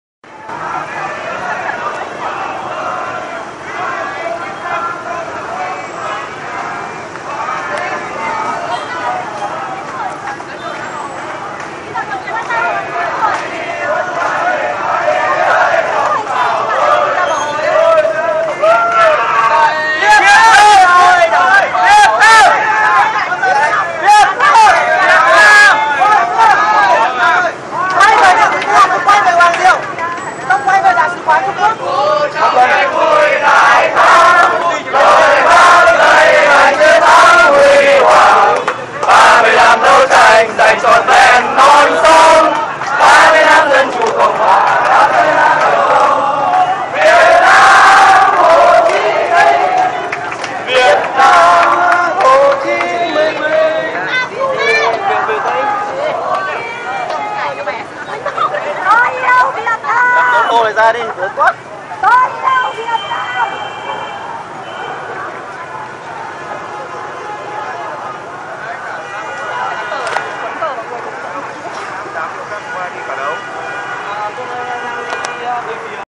Biểu tình ôn hòa phản đối Trung Quốc chủ nhật 5/6/2011
Đây là cảnh thanh niên già trẻ khắp nơi mang cờ và biểu ngữ nô nức đi biểu tình phản đối việc Trung Quốc xâm phạm khu vực biển đông của Việt Nam, sáng chủ nhật 5/6/2001.

Tags: bieu tinh chong trung quoc xam luoc, bieu tinh phan doi trung quoc

Clip của dungtranglobal